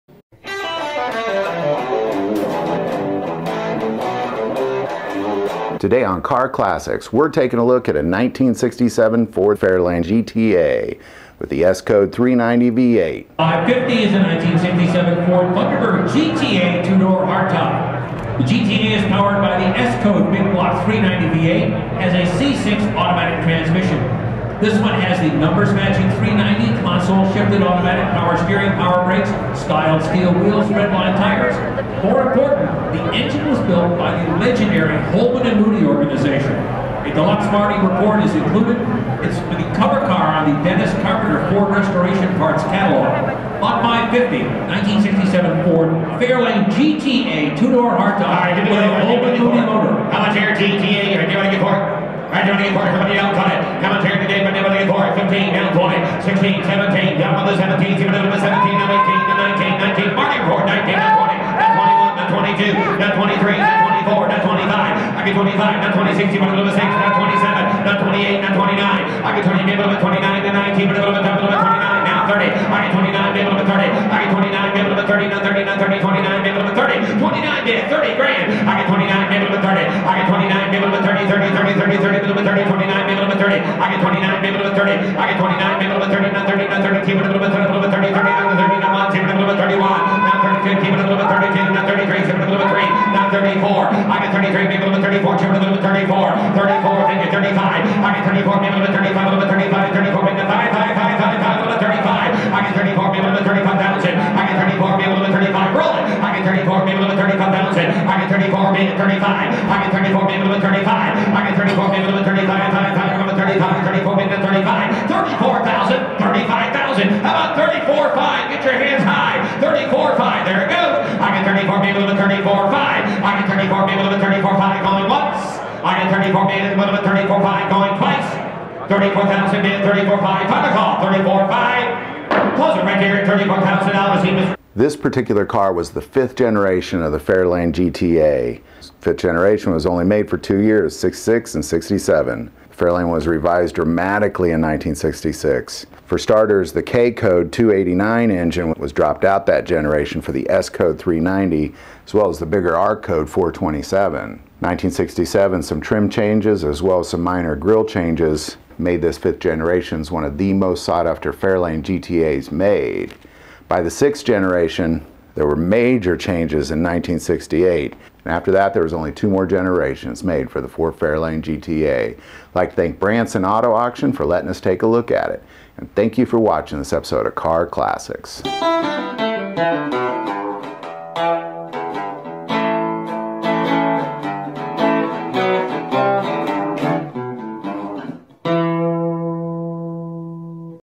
Classic Car Auction : 1967 Ford Fairlane GTA
This video takes a look at a 1967 Ford Fairlane GTA as it goes through auction.